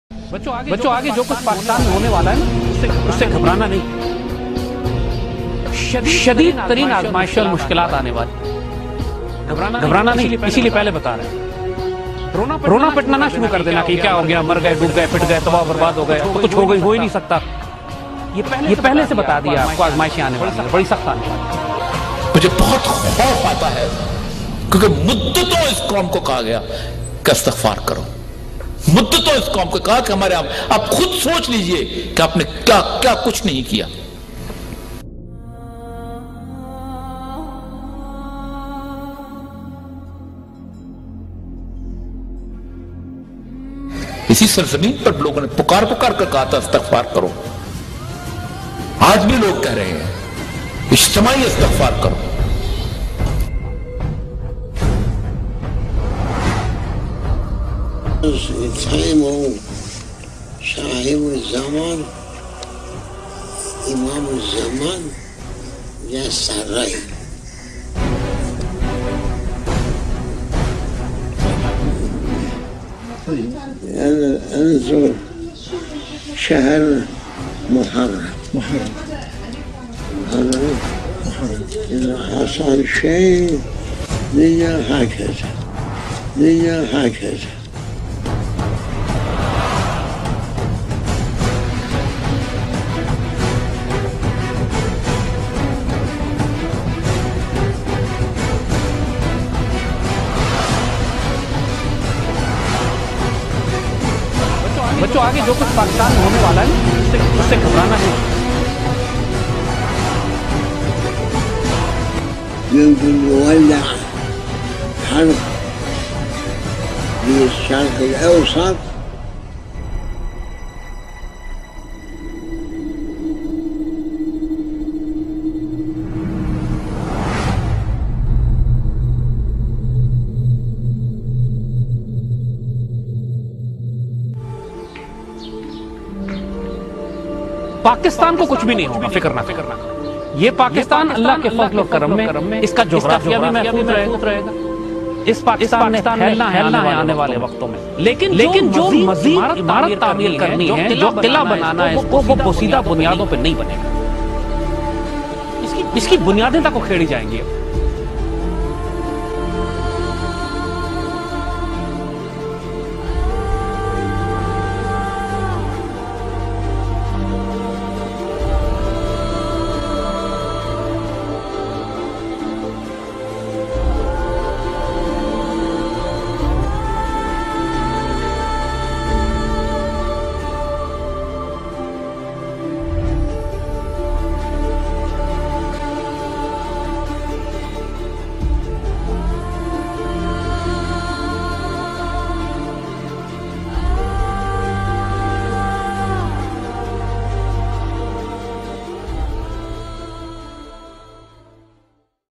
Heed These Warnings - The Time is almost OVER
Heed these warning. Warnings had been coming from all faqeers from last few months, nobody had ever thought the things would change this much faster. There are serious chances of third world war getting start. We must prepare ourselves for everything whats coming physically, mentally and spiritually.